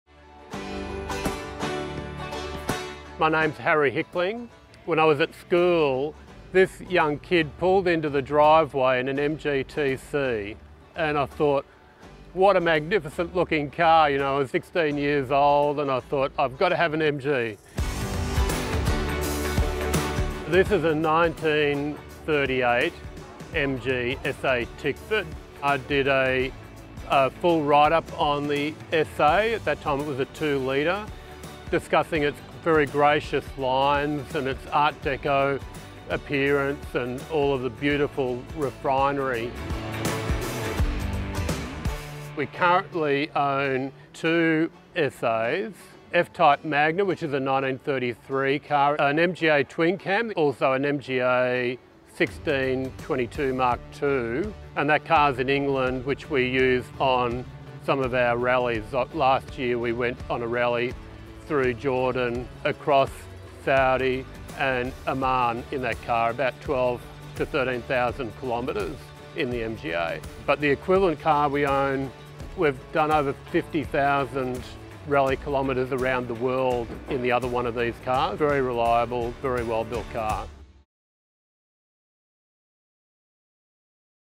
Moments with MG Custodians | Harry Hickling
Harry Hickling’s passion for MGs ignited at 16 when a classmate drove up in an MGTC. Since then, he’s built an impressive collection of heritage MG vehicles, taking them on extraordinary global road trips.

At the MG Car Club National Meeting in Tamworth, Harry proudly displayed his 1938 MG SA Tickford, with its stunning art deco design.

Here’s to 100 Years of MG and the enthusiasts who keep its legacy alive through exploration and celebration.